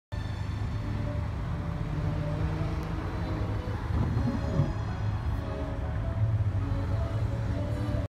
Nice
double rainbows in California during sunset